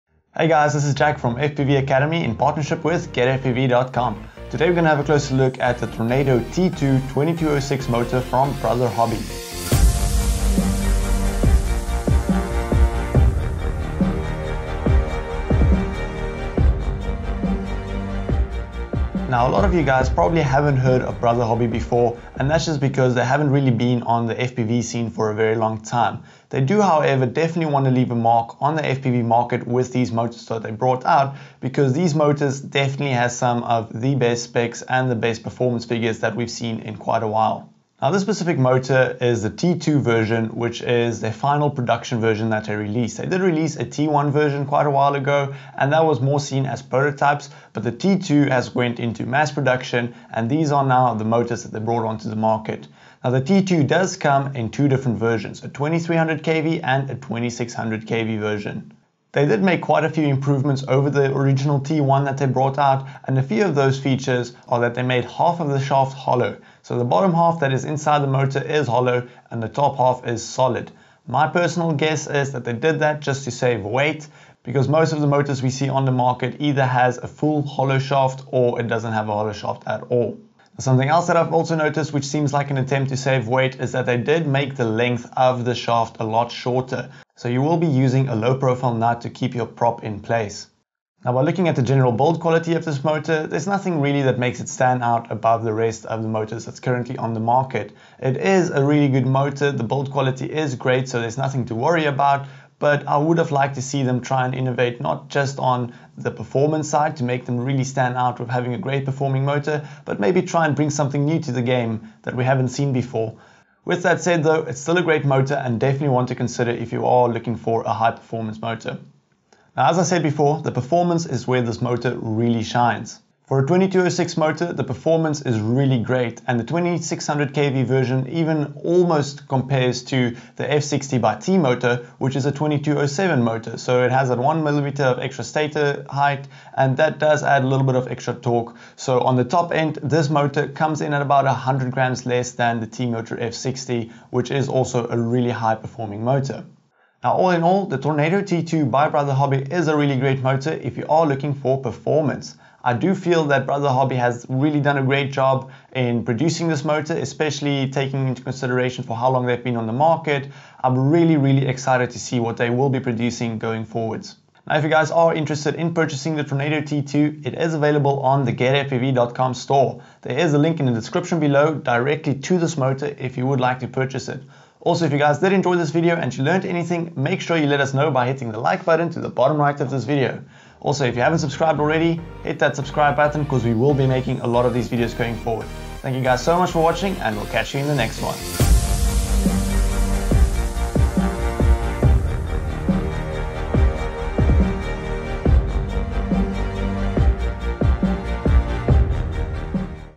Tornado T2 2206 // FPV Motor // GetFPV.com
Hey FPV Community!

In this video, Jack will do a general overview of the Tornado T2 Motor by Brotherhobby.